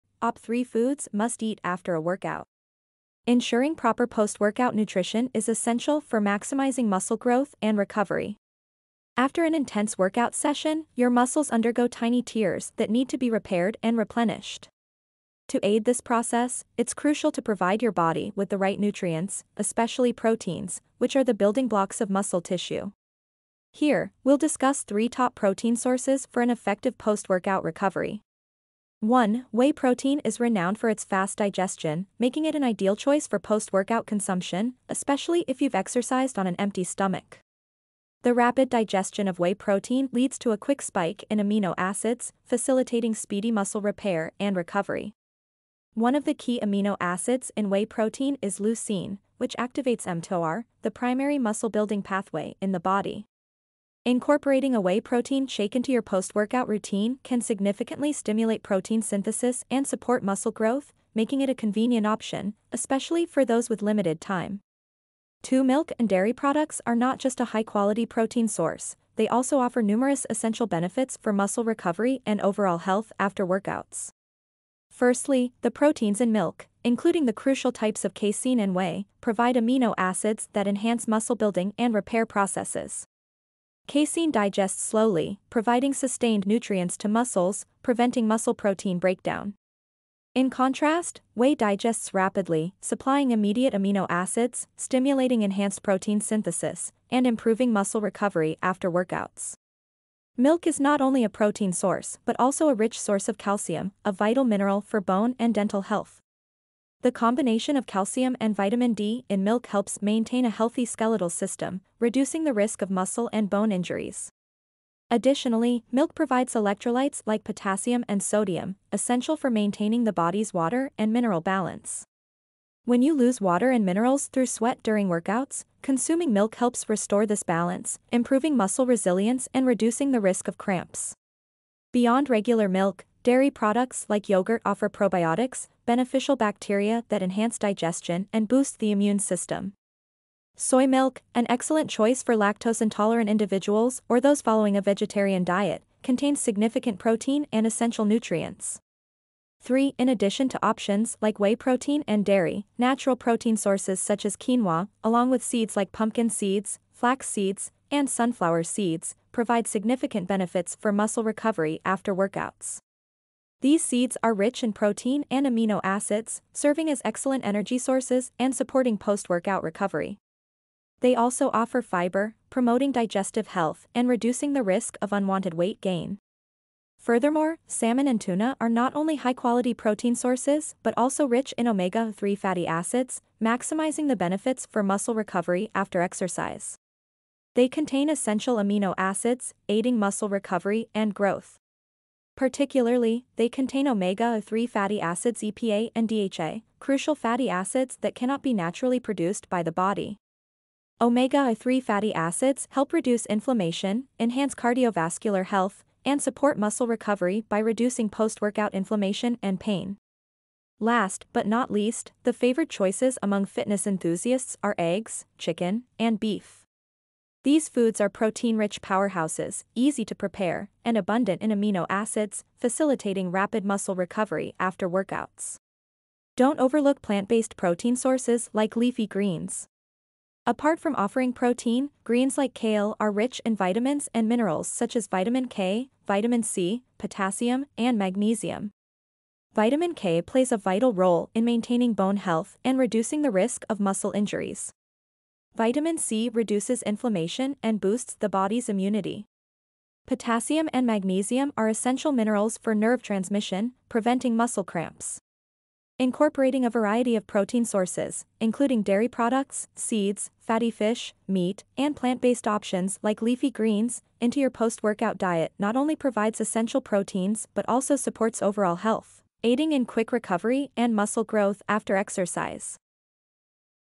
Top 3 Foods Must Eat After a Workout
Ensuring proper post-workout nutrition is essential for maximizing muscle growth and recovery. After an intense workout session, your muscles undergo tiny tears that need to be repaired and replenished. To aid this process, it's crucial to provide your body with the right nutrients, especially proteins, which are the building blocks of muscle tissue. Here, we'll discuss three top protein sources for an effective post-workout recovery.